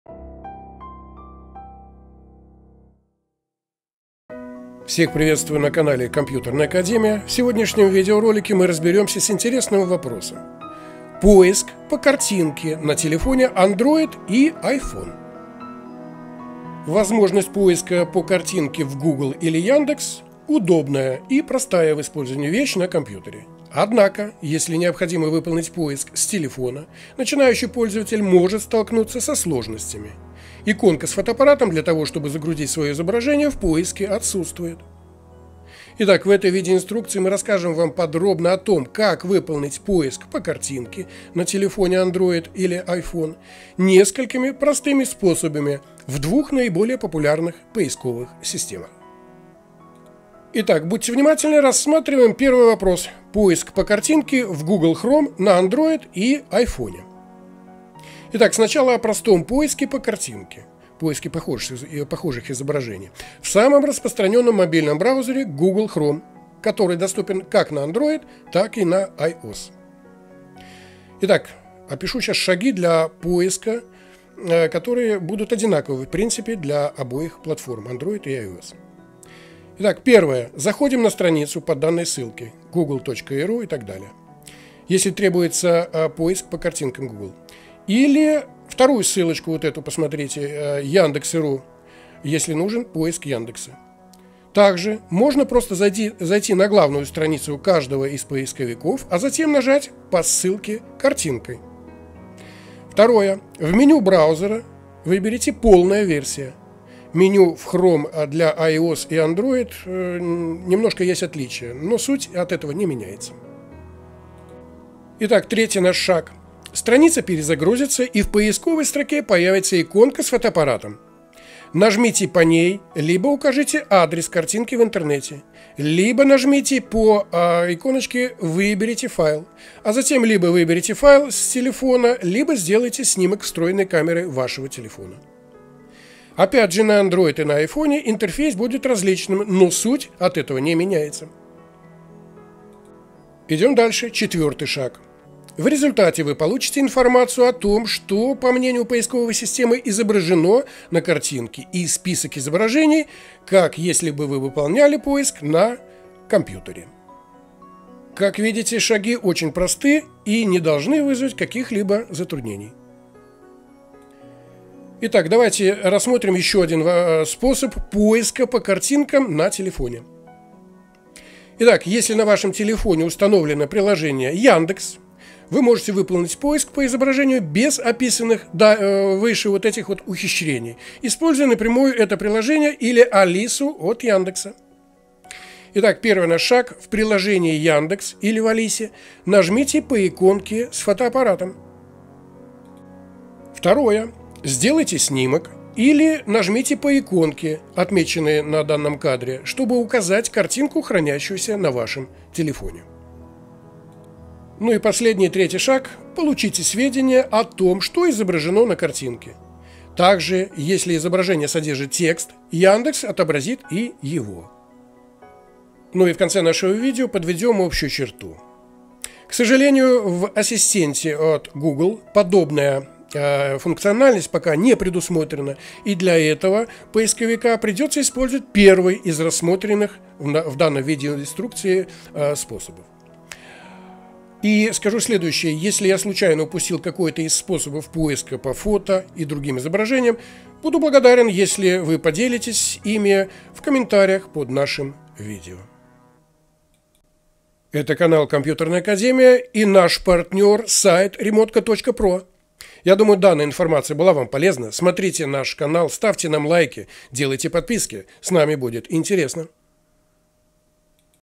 Поиск по картинке на телефоне Android и iPhone
Возможность поиска по картинке в Google или Яндекс — удобная и простая в использовании вещь на компьютере, однако, если необходимо выполнить поиск с телефона, начинающий пользователь может столкнуться со сложностями: иконка с фотоаппаратом для того, чтобы загрузить своё изображение в поиск отсутствует.

В этой инструкции подробно о том, как выполнить поиск по картинке на телефоне Android или iPhone несколькими простыми способами в двух наиболее популярных поисковых системах.

Поддеджите наш канал  Visa Ощадбанк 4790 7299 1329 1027  - Блин ... а жить то надо

- Наша группа Вконтакте, вступайте и приглашайте друзей.